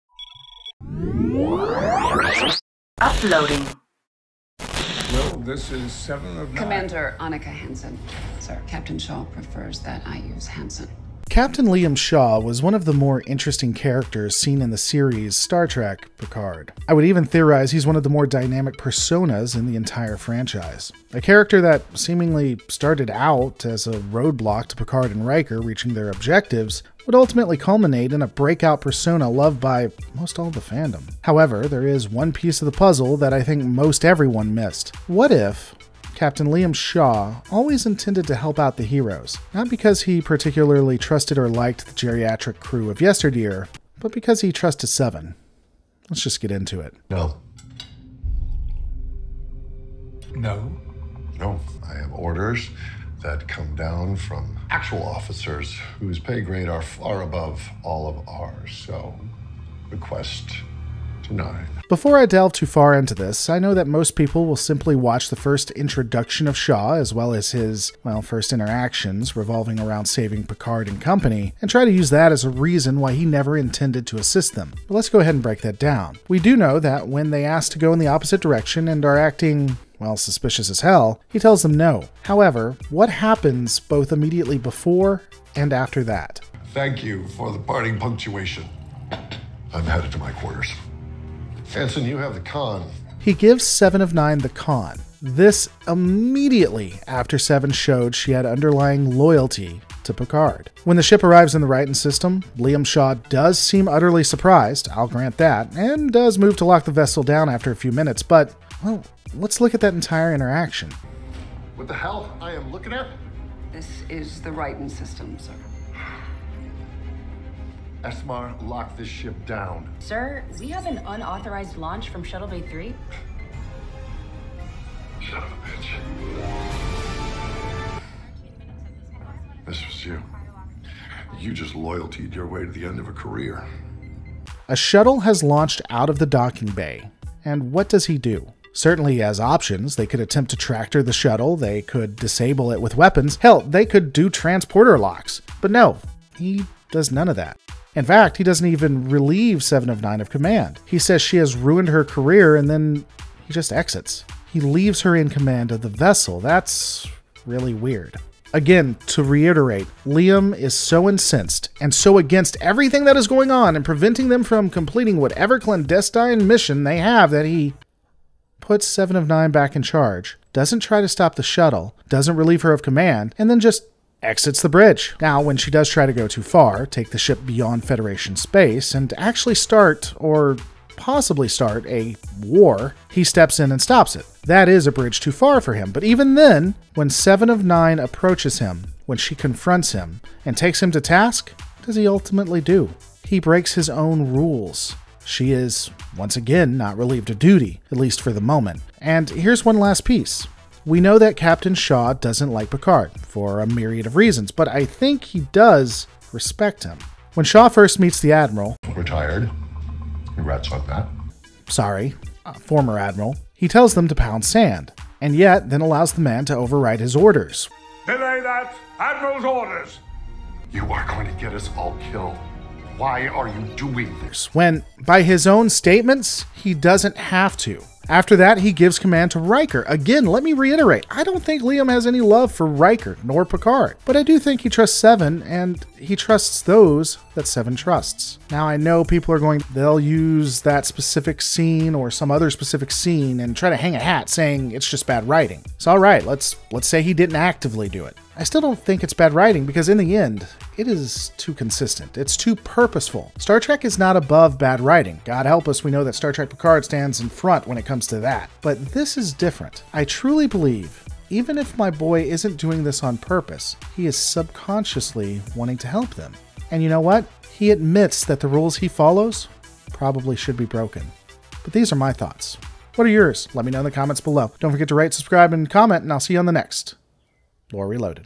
Captain Shaw's Secret that most all fan's missed (Star Trek)
Let's Just Get into it..

00:00 - SPOILERS
00:10 - Intro
00:47 - Shaw's Resistance

Want to see something live? Check out..

2441-Q Old Fort Pkwy. #343